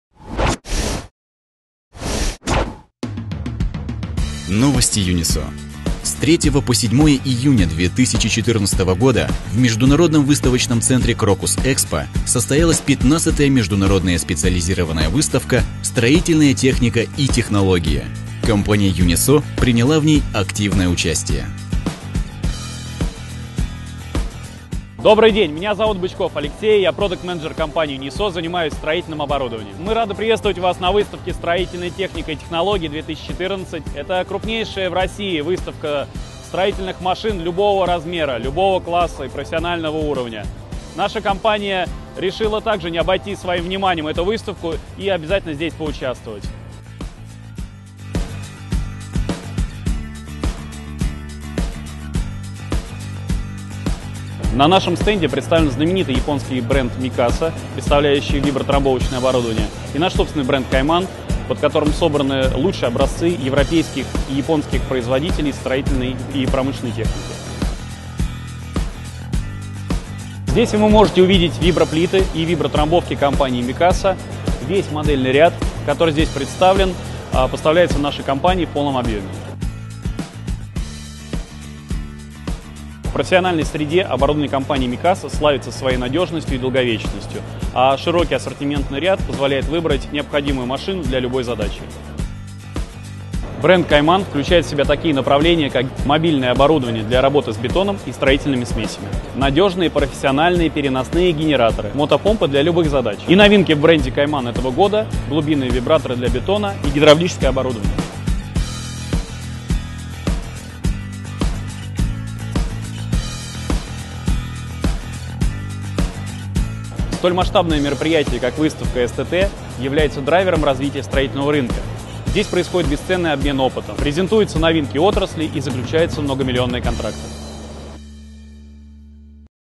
Новая строительная выставка!!! Техника и Технологии - СТТ 2014
3 июня 2014 года в Крокус-Экспо начала работу 15-я Международная специализированная выставка "Строительная Техника и Технологии'2014".